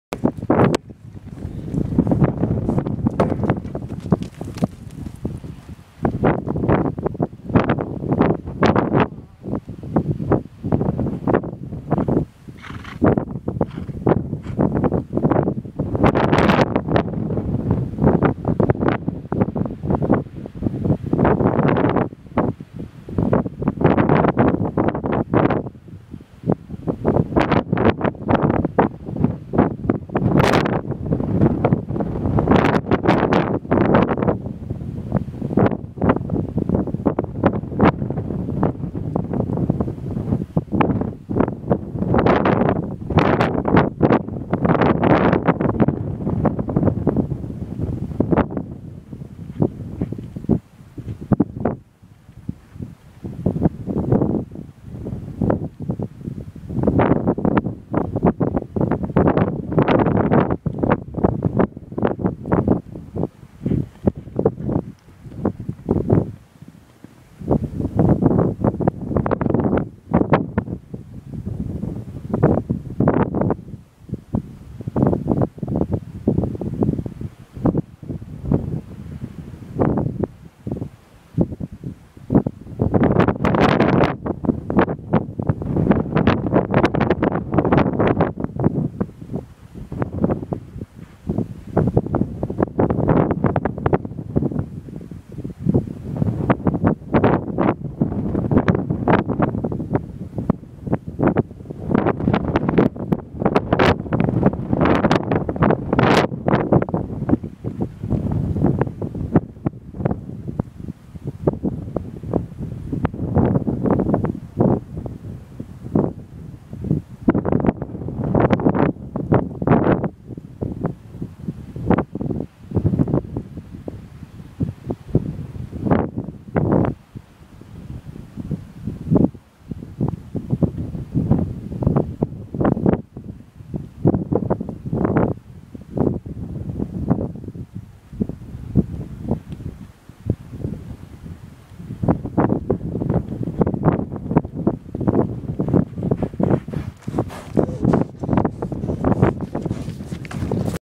Teaching my 3 yrold Pete poles
Beginning of 2011 teaching my 3 year old Pete how to run poles!! Doing alright for the beginning!!